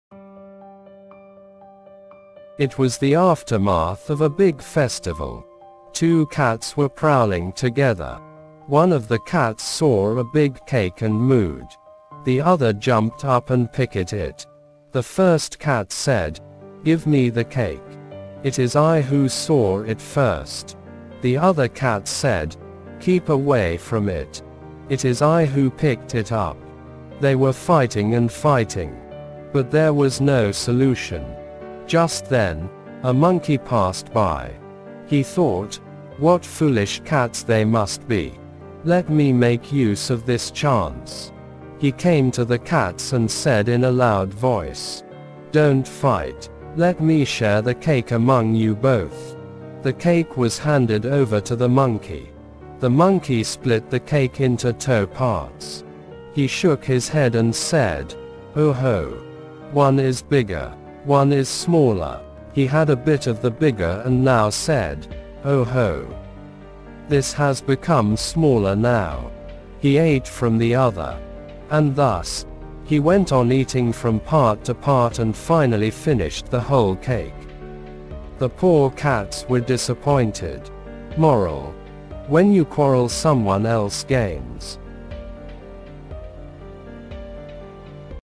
Learn English through Stories | Animal Story | Story 304 | The Monkey and Cats
EGOLISH is a platform to practice English language with wonderful stories.